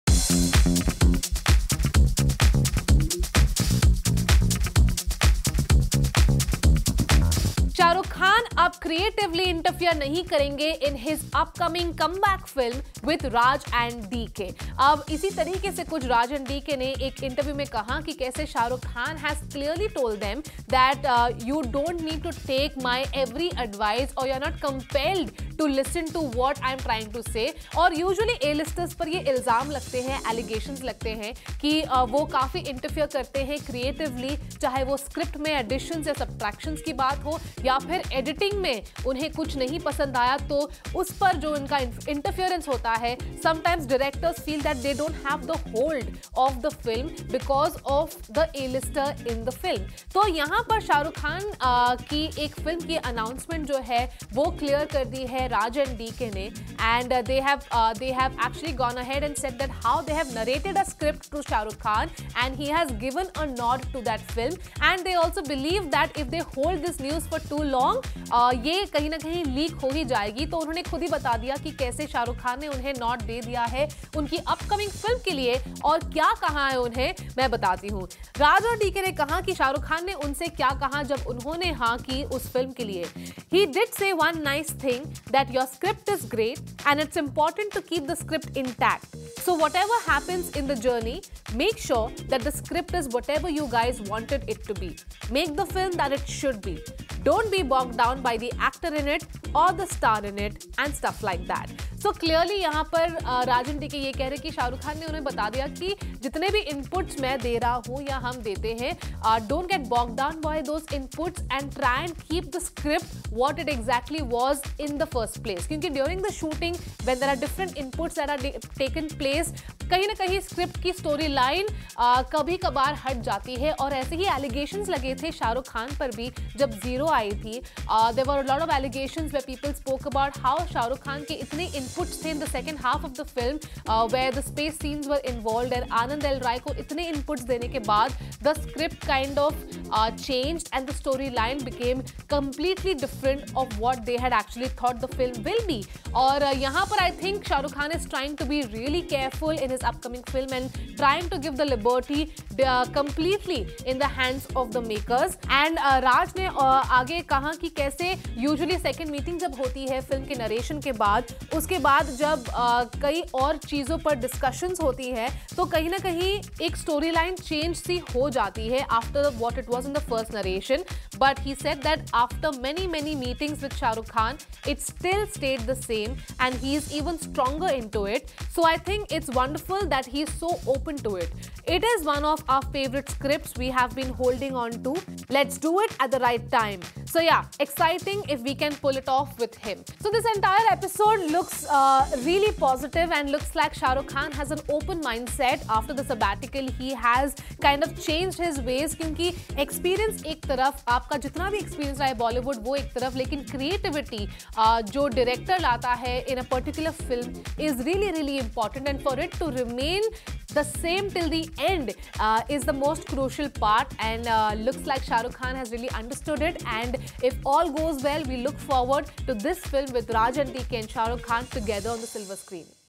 Shah Rukh Khan's comeback film CONFIRMED by Raj and DK | Bollywood News
Shah Rukh Khan who has been on a break since his last film Zero has finally confirmed his comeback film as revealed by Raj and DK. Raj and DK in an interview said that Shah Rukh Khan has liked one of their scripts and told them that he would not interfere in the creative side of making the film. Watch the video to know more.